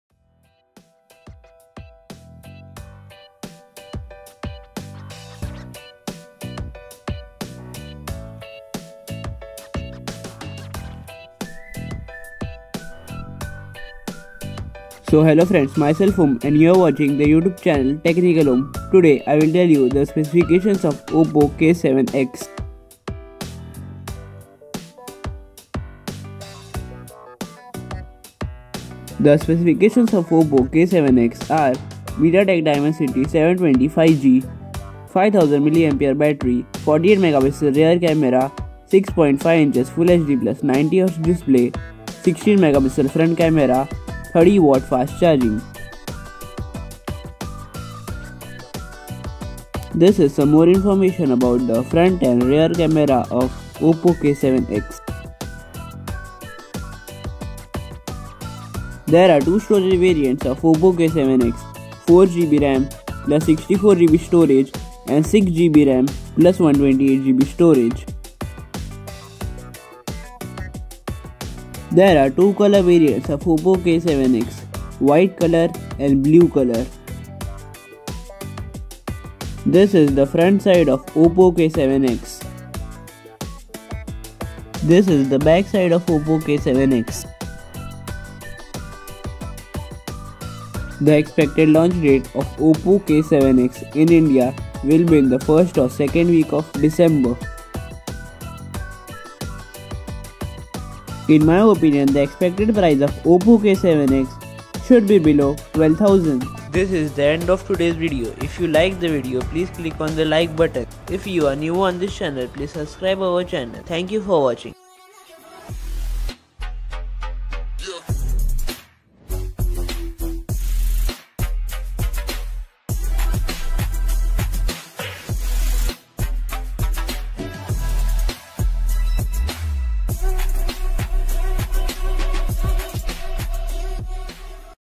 Oppo K7x | Full Specifications 🔥🔥 | MediaTek Dimensity 720 | 48MP | 5G | Price ⚡
Hello Friends Today I will tell you the Specifications and expected price of Oppo K7x

Technical Om

Video Highlights

00:00 Introduction